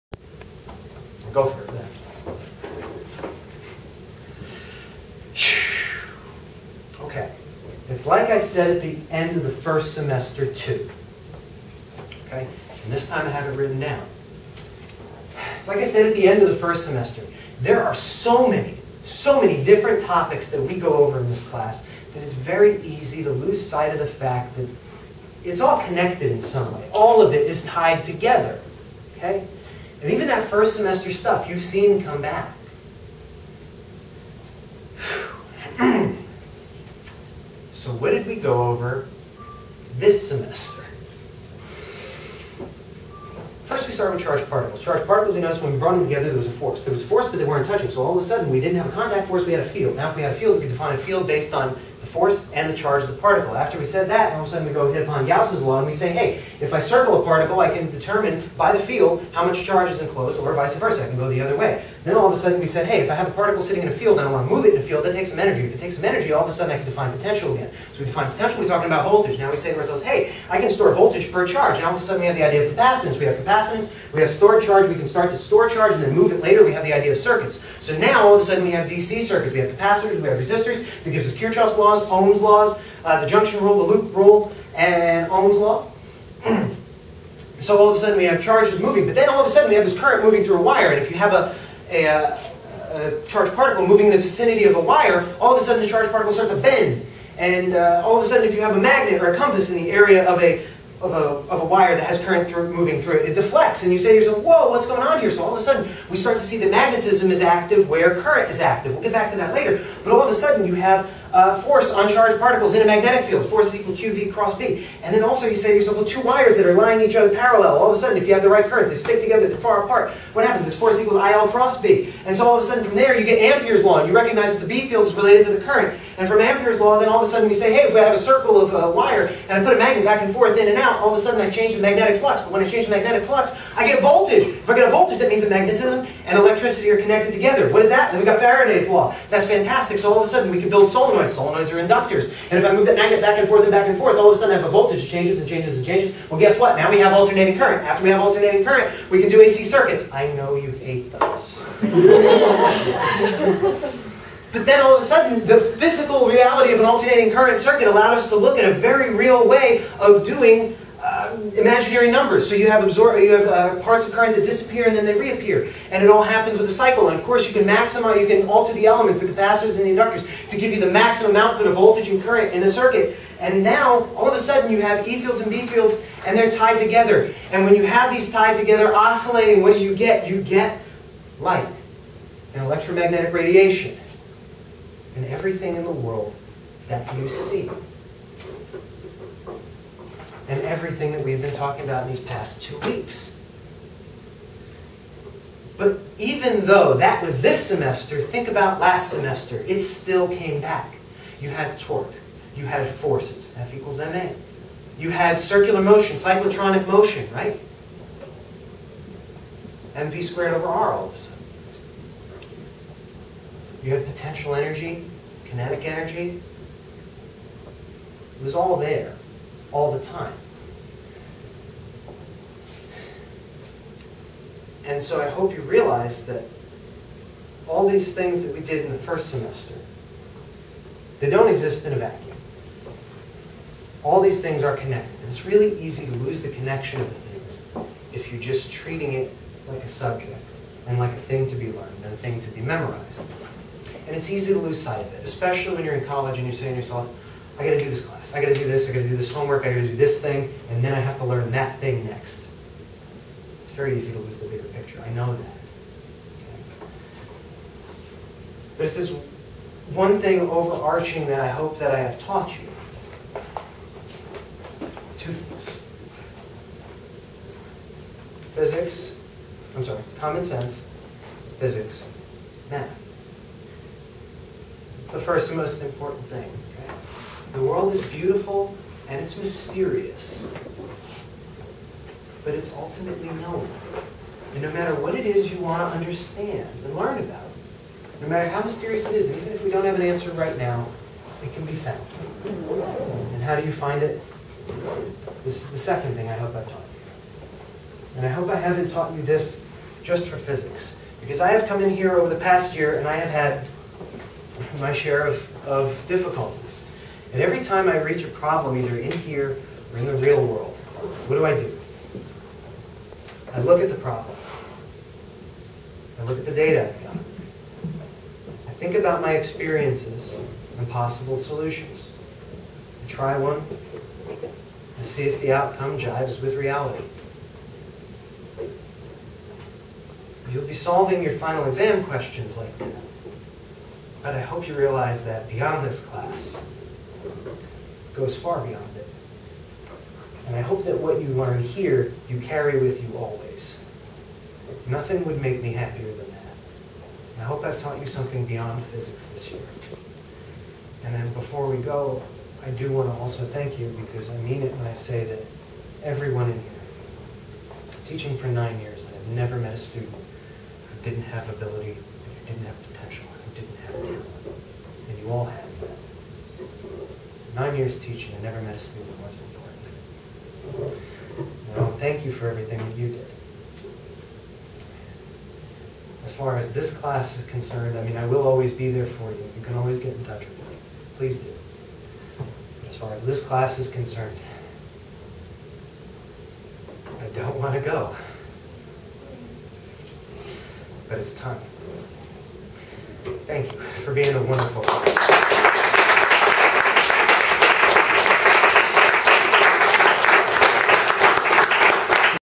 Dr. La Porta's Final Physics 104 Precept at Princeton
Today was the last precept of the semester.  Summarized the topics from the semester and then talked about the real lessons Physics has to teach us. Yes, I teared up when I said good bye.